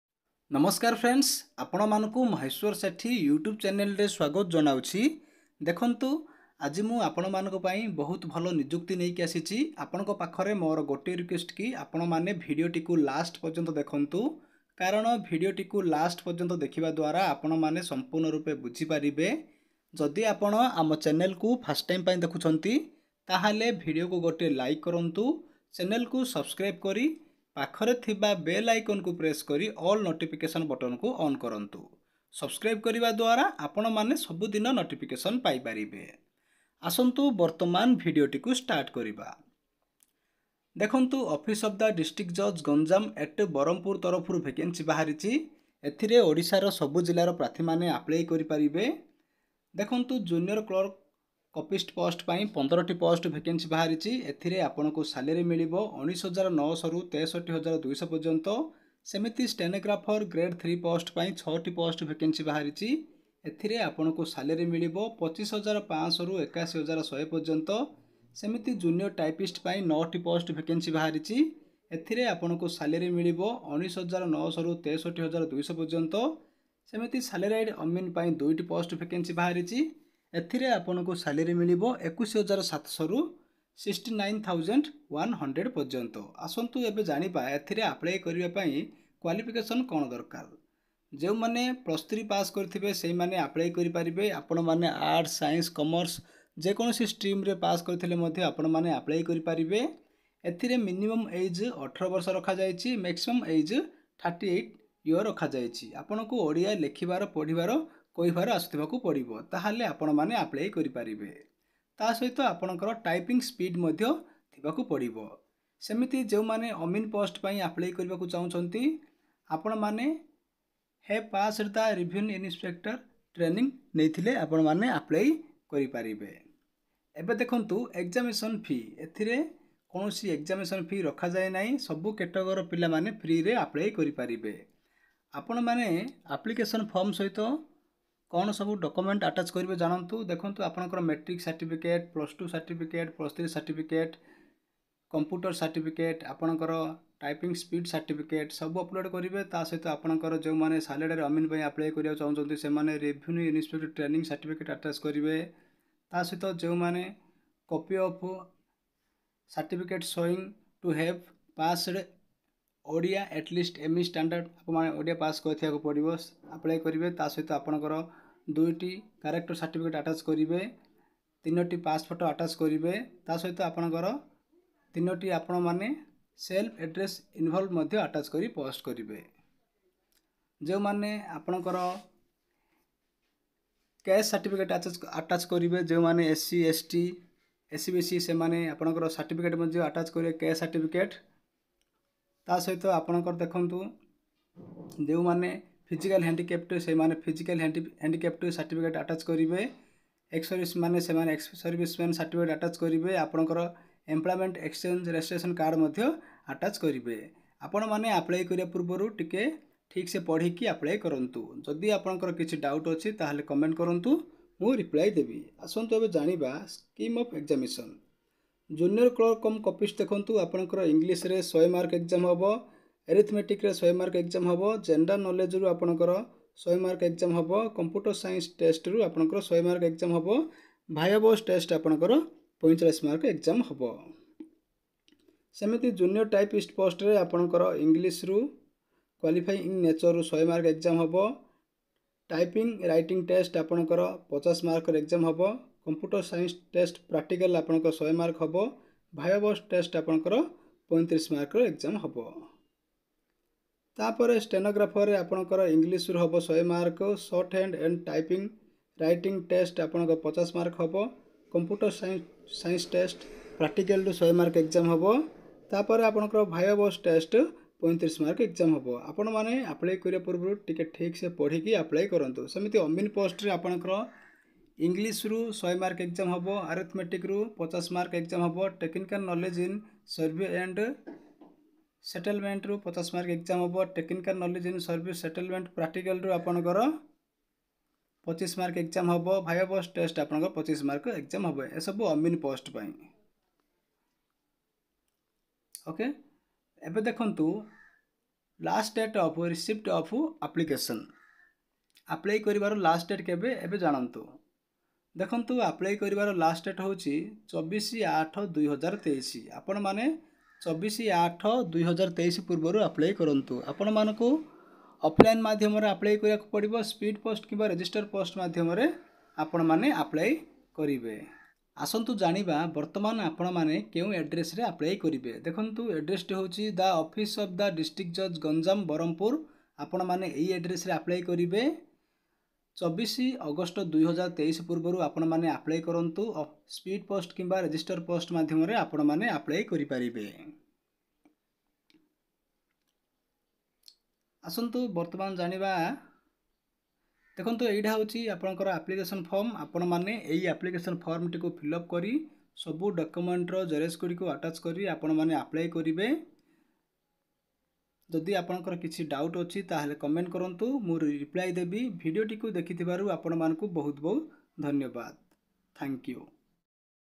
DISTRICT COURT ରେ JUNIOR CLERK POST ପାଇଁ ନିଯୁକ୍ତି | SALARY:- 25,500 /- | APPLY କରିବା ପାଇଁ FREE
District Court Vacancy 2023.

Odisha District Court Recruitment 2023.

Junior Clerk & Junior Typist Vacancy 2023.

Stenographer & Amin Recruitment 2023.

MAHESWAR SETHI